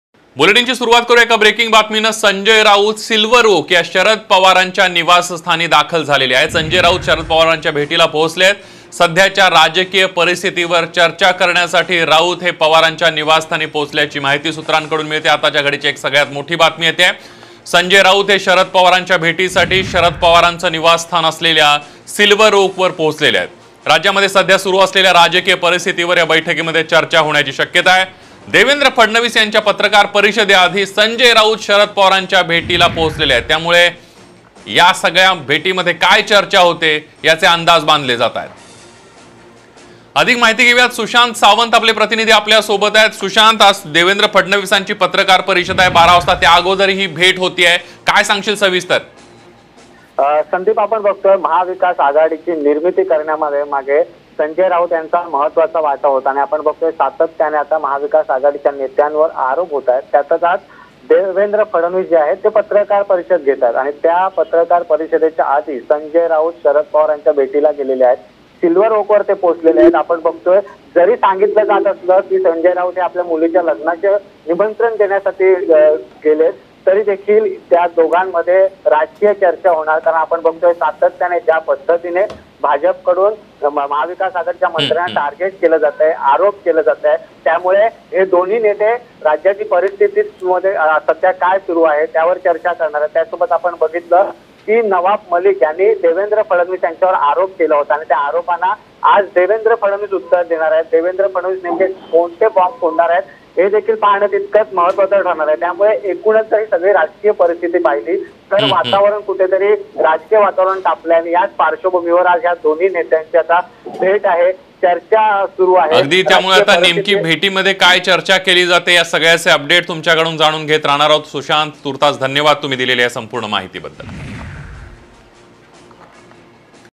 Sanjay Raut: संजय राऊत सिल्व्हर ओकवर दाखल | Sakal Media
संजय राऊत सिल्व्हर ओकवर दाखल, सध्याच्या राजकीय परिस्थितीवर चर्चा-सूत्र